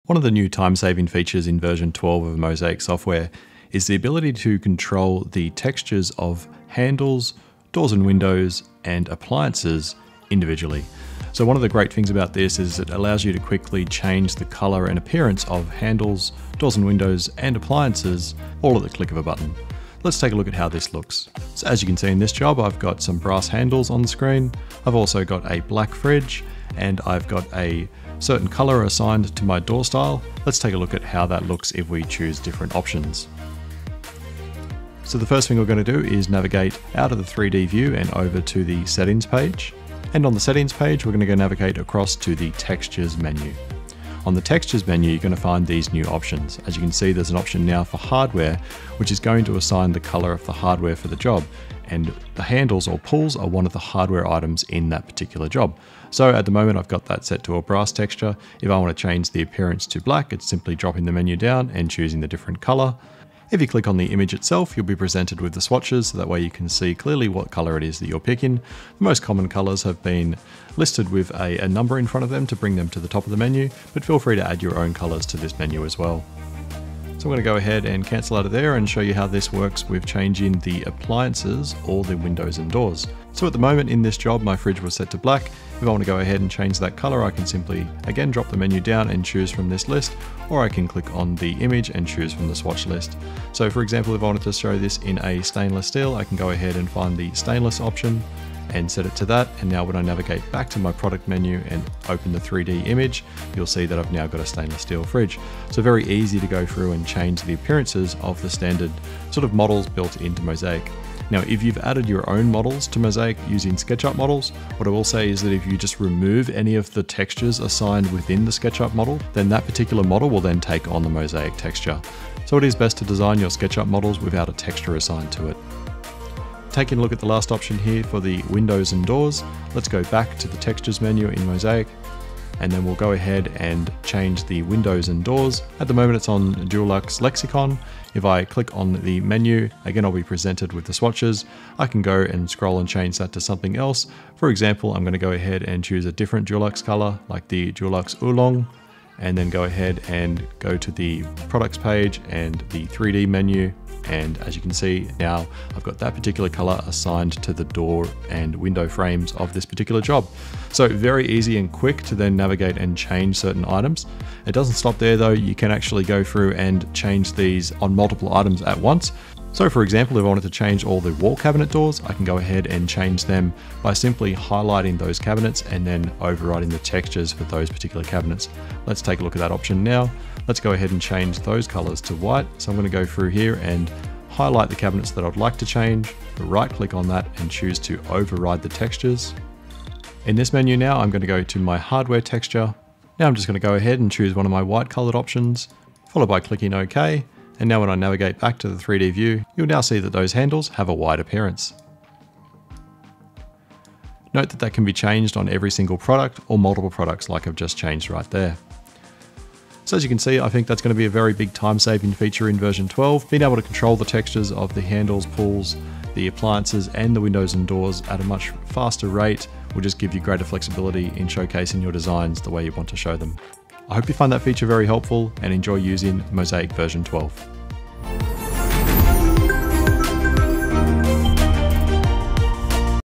Mozaik Software Version 12 New Feature: New Texture Settings
Mozaik Software Version 12 New Feature!

The option to control the colour/appearance of hardware, appliances and windows and doors independently is a big time saver for anyone needing to create professional 3D drawings.